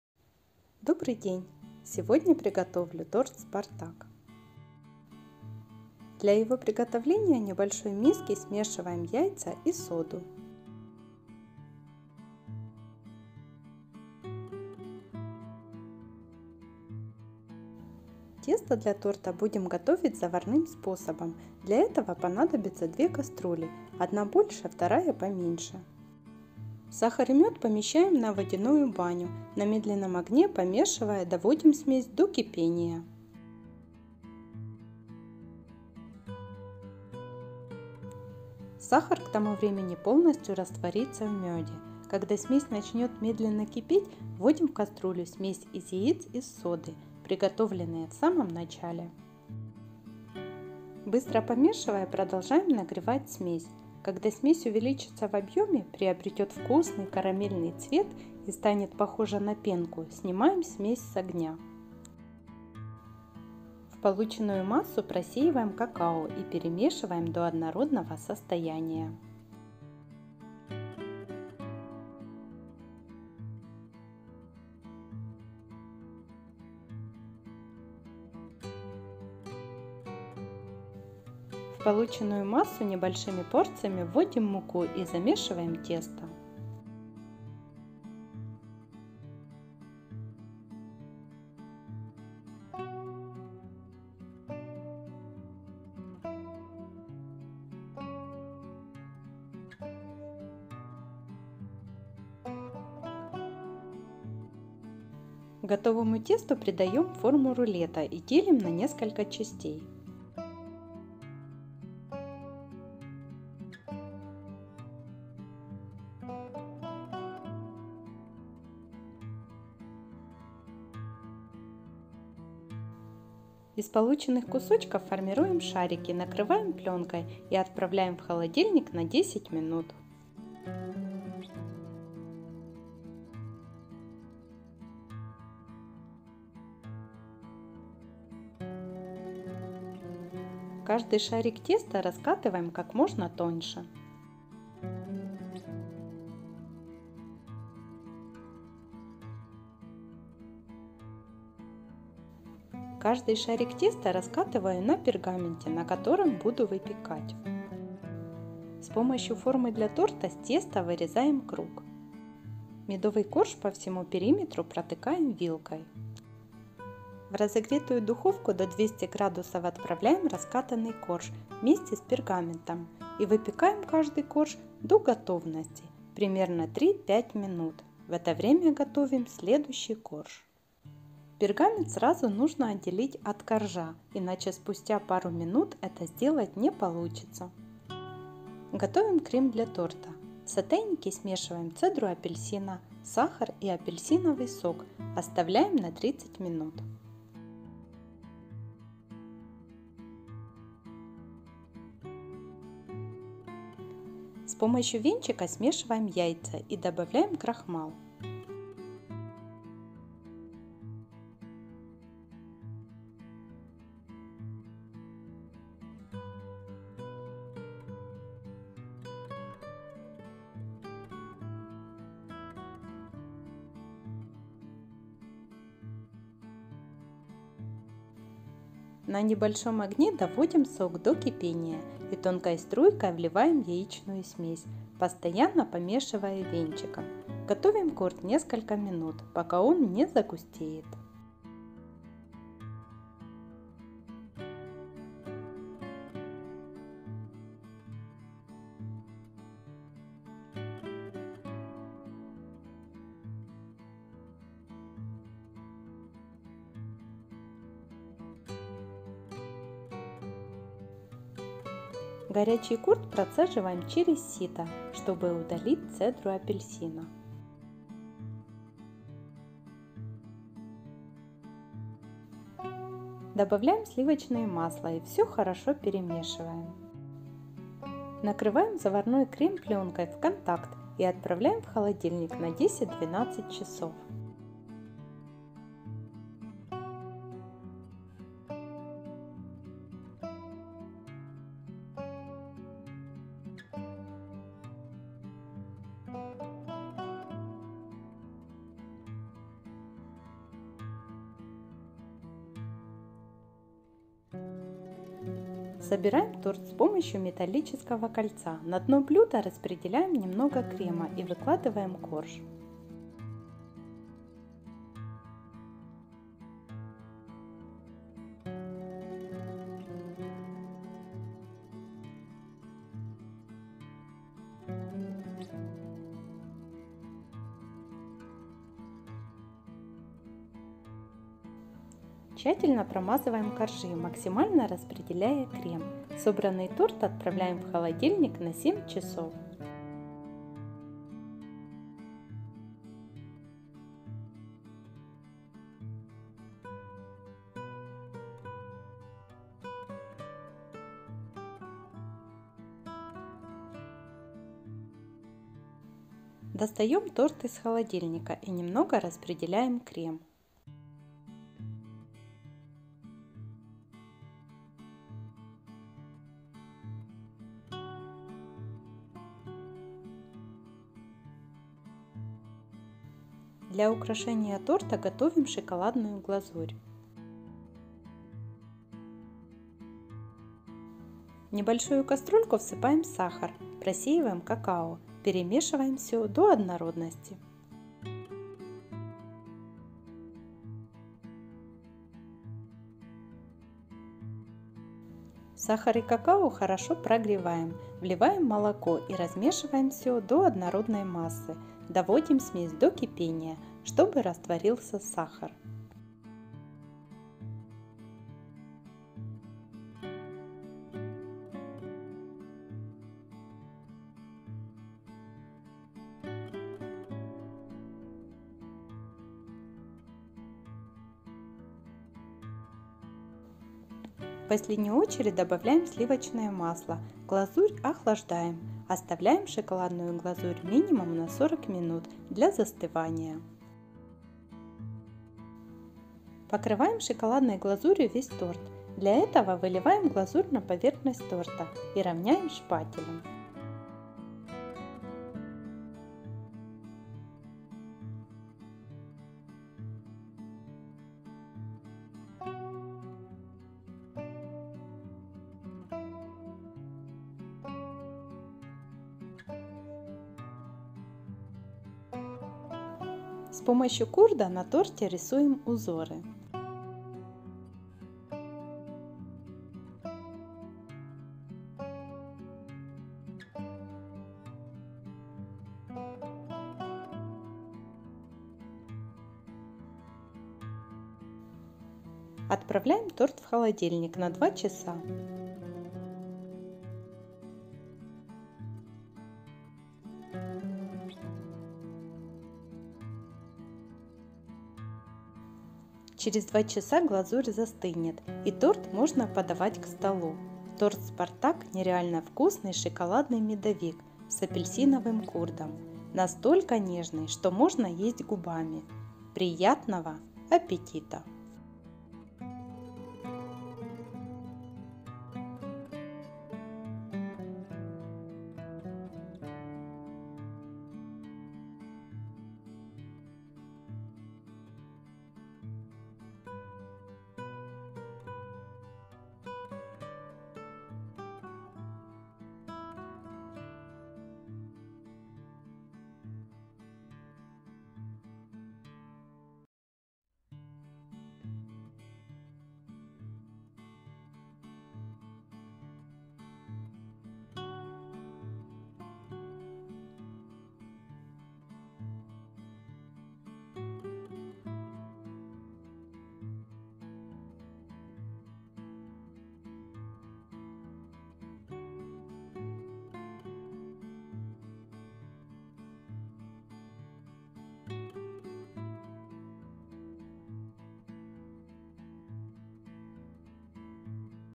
Простой Быстрый ШОКОЛАДНЫЙ ТОРТ/КУЛИНАРИЯ/Chocolate Cake Recipe СМАЧНИЙ РЕЦЕПТ ГОТОВИМ ВКУСНО
ДОБРЫЙ ДЕНЬ!

    ЗДЕСЬ Я ДЕЛЮСЬ СВОИМИ РЕЦЕПТАМИ И ИДЕЯМИ В ОФОРМЛЕНИИ ПРАЗДНИЧНОГО СТОЛА.
ВСЕМ ДОБРА.

мед 4стл
яйца 3шт
мука 4 ст 520 г
сахар 2 ст 360г
сода 1чл
како 60г (4ст л)
 апельсиновый курд
апельсиновый сок 800мл
цедра апельсина 2шт
сахар 6стл
крахмал 6стл
яйцо 2шт+желток
сливочное масло 50г
шоколадная глазурь
какао 3стл
сахар 100г
молоко 50мл